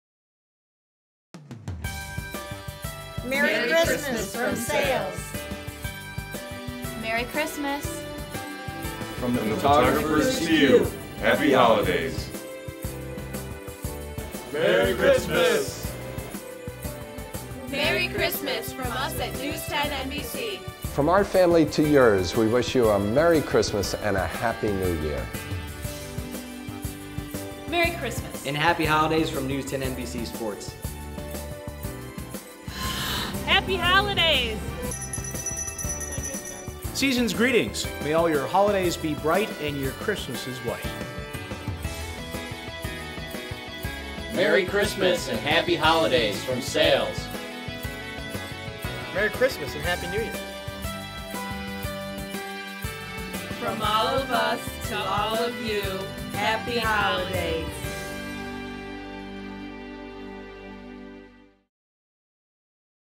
2012 WHEC Christmas Greetings
One of the employee Christmas greeting spots running on WHEC